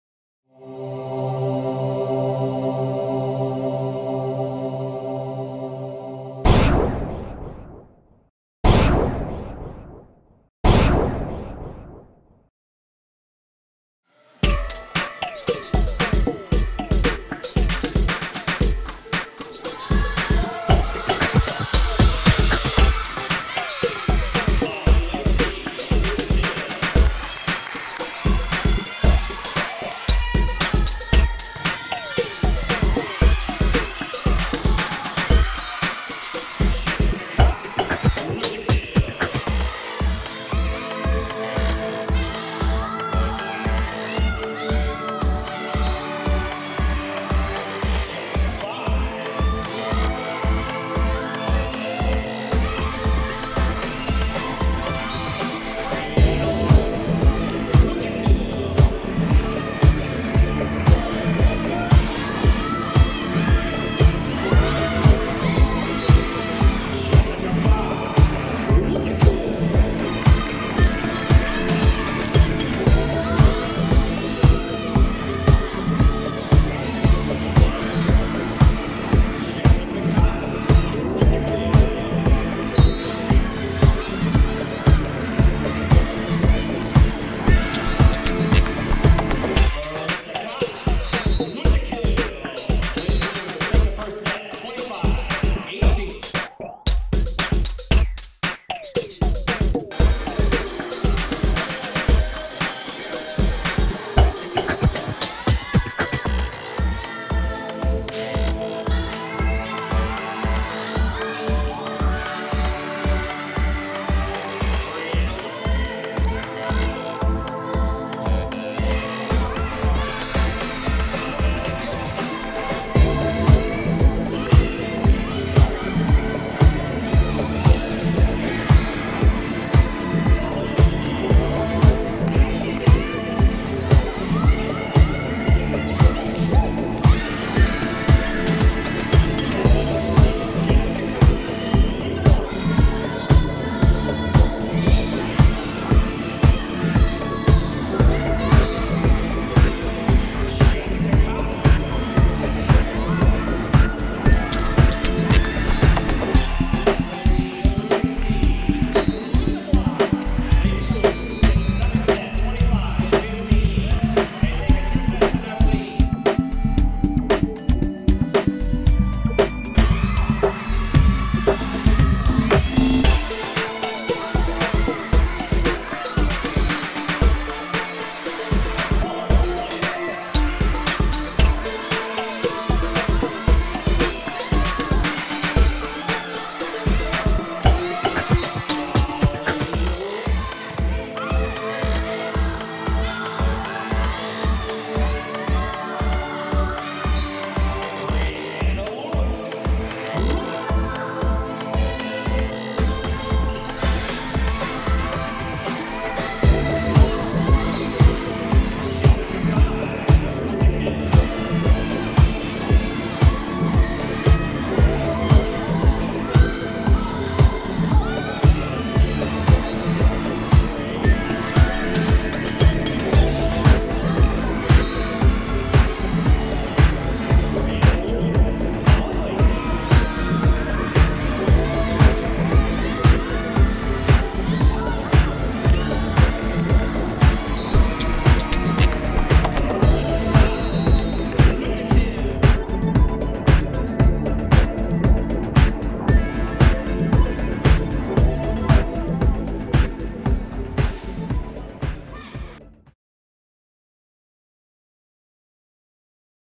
WUSPORTS: Lady Blues vs. NWMSU - Volleyball
Washburn Lady Blues vs. Northwest Missouri State - Volleyball. October 25, 2011. Music by danosong.com